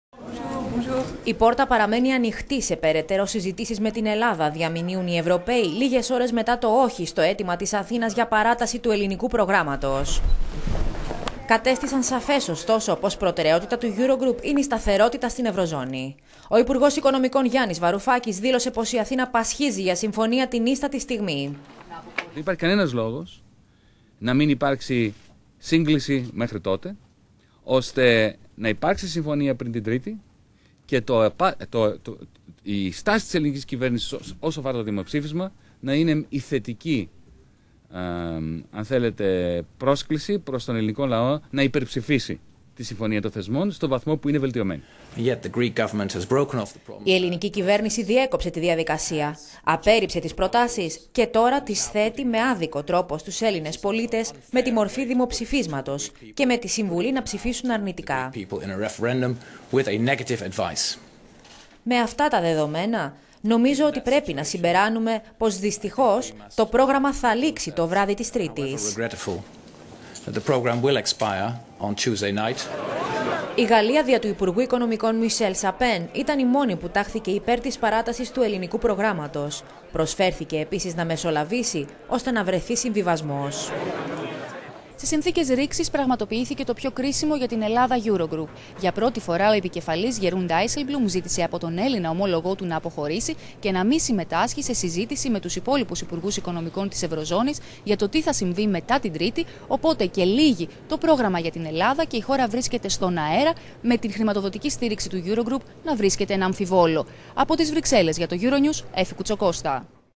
Eurogroup: «Όχι» σε παράταση, «ναι» σε συζητήσεις
Η πόρτα παραμένει ανοιχτή σε περαιτέρω συζητήσεις με την Ελλάδα, διαμηνύουν οι Ευρωπαίοι λίγες ώρες μετά το «όχι» στο αίτημα της Αθήνας για παράταση του ελληνικού προγράμματος.

Κατέστησαν σαφές, ωστόσο, πως προτεραιότητα του Eurogroup είναι η σταθερότητα στην ευρωζώνη.

Ο υπουργός Οικονομικών Γιάνης Βαρουφάκης δήλωσε πως η Αθήνα πασχίζει για συμφωνία την ύστατη στιγμή:
«Δεν υπάρχει κανένας λόγος να μην υπάρξει σύγκληση μέχρι τότε, ώστε να υπάρξει συμφωνία πριν την Τρίτη,
και η στάση της κυβ…